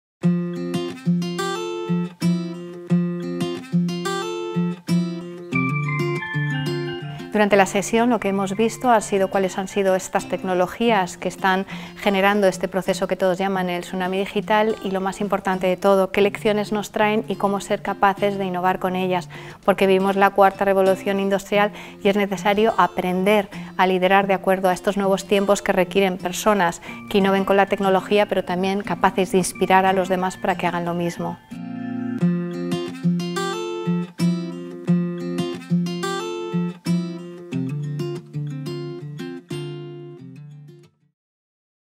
El ABC de la Estrategia - Silvia Leal - e-Liderazgo
(17-12-18).- Silvia Leal, consejera de Innovación en la Comisión Europea, explica las claves de su sesión en El ABC de la Estrategia de EDEM, "e-Liderazgo".